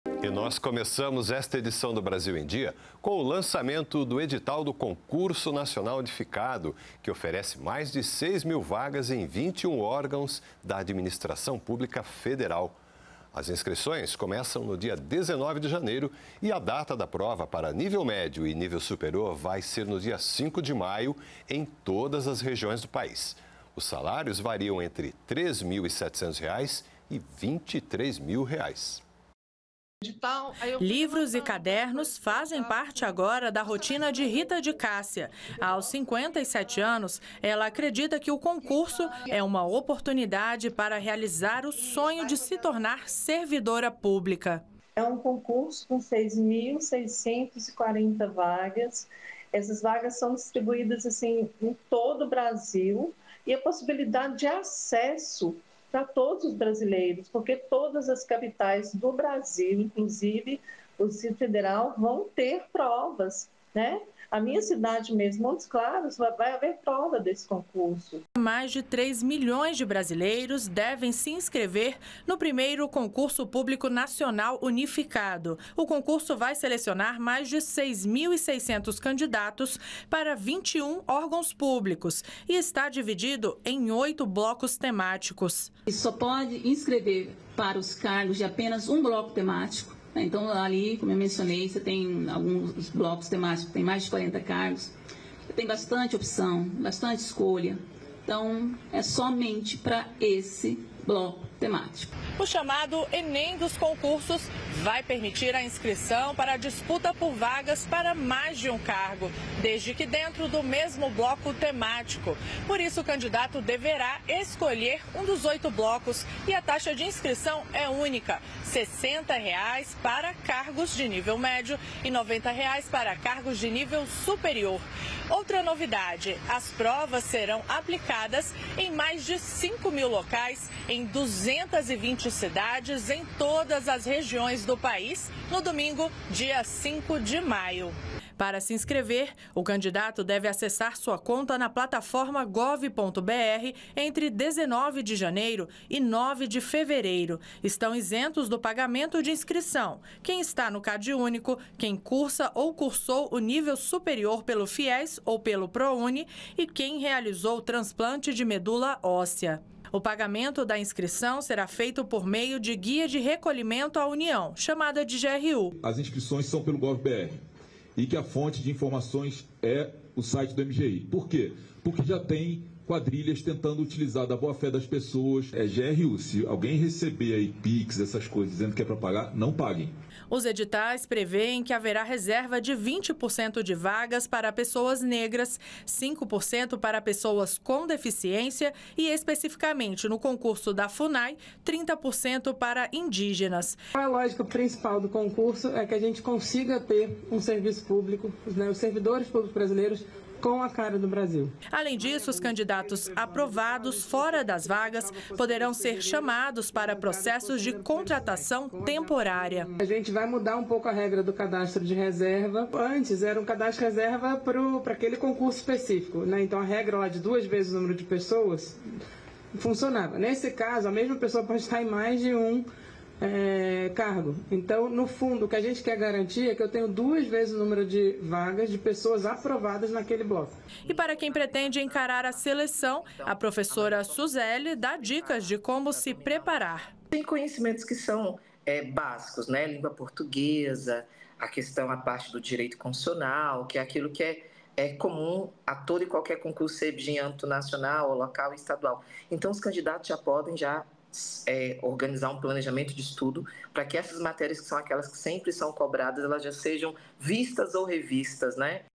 GOVERNO LANÇA EDITAL PARA CONCURSO PÚBLICO COM MAIS DE 6 MIL VAGAS
O governo lançou o edital do Concurso Público Nacional Unificado, que oferece 6.640 vagas para órgãos federais. As inscrições começam em 19 de janeiro, e a prova está prevista para 5 de maio. Os salários variam entre R$ 3.700 e R$ 23 mil.